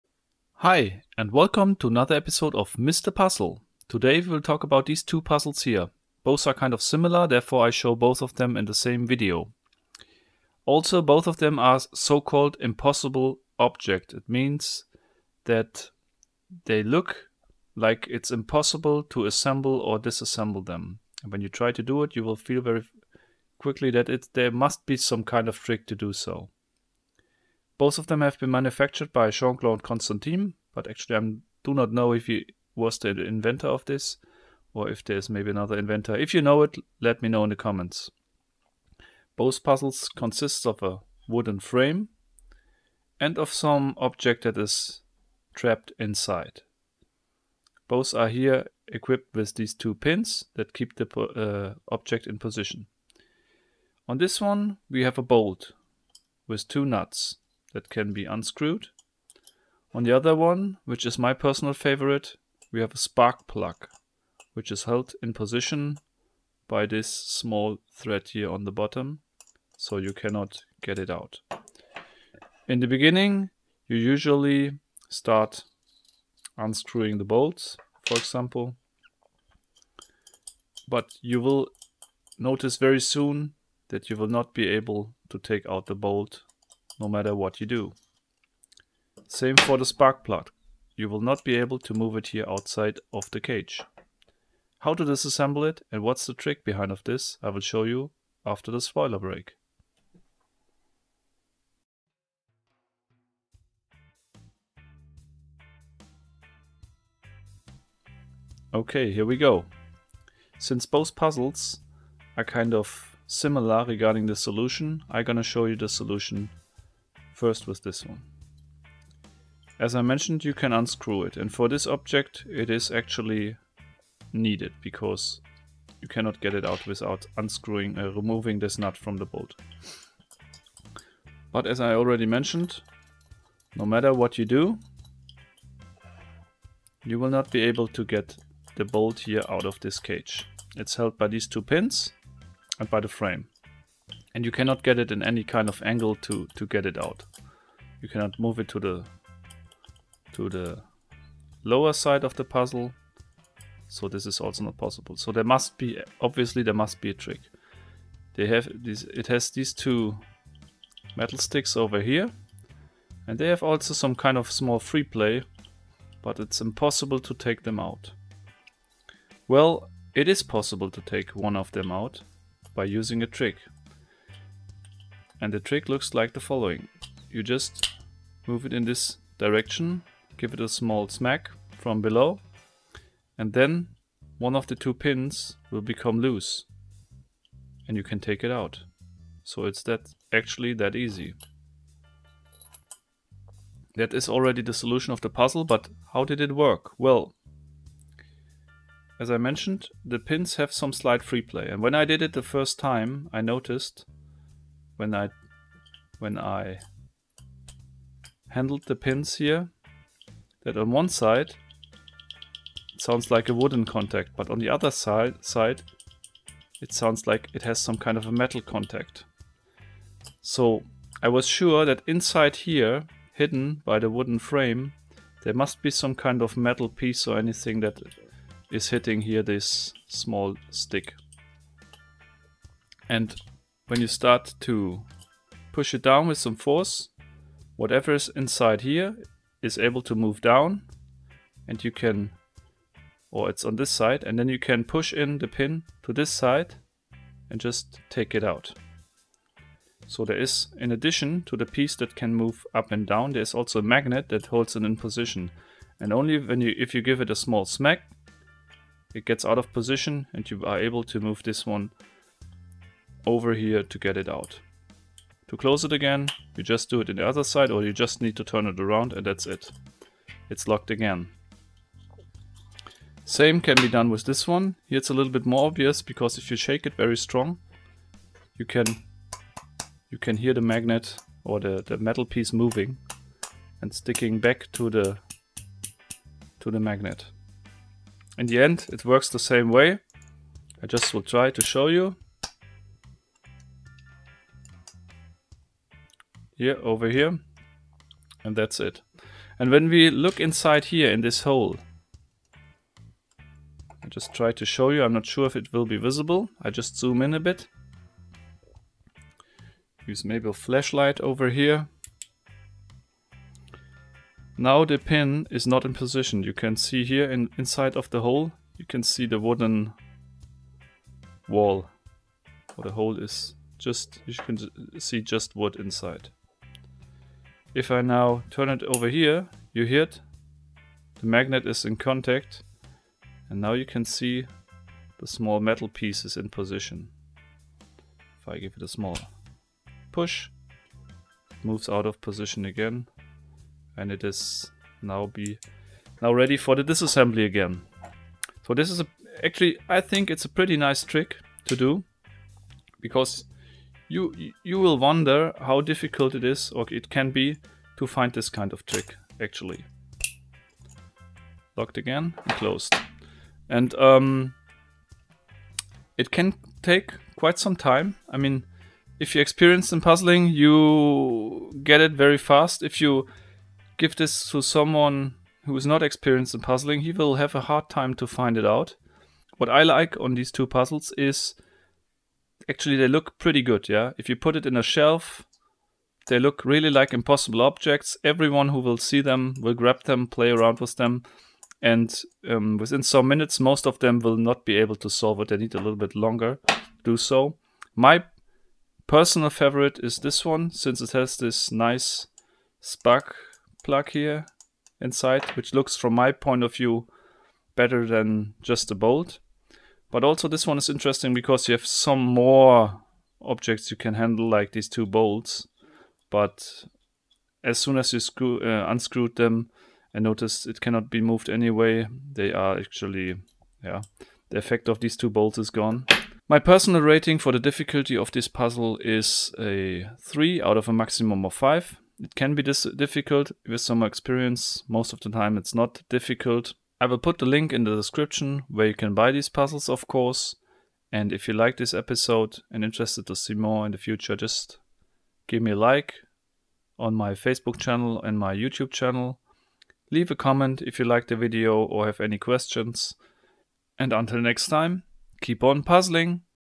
The Impossible Spark Plug and Bolt - How to get them out!
Two puzzles similar solution. Another mind boggling, impossible looking eye-catcher made by Jean Claude Constantin. Can you imagine how to get the spark plug out of the frame? One is always wiser after seeing the solution but in reality, it's tricky even if you have the right intention. In this video, I explain you why.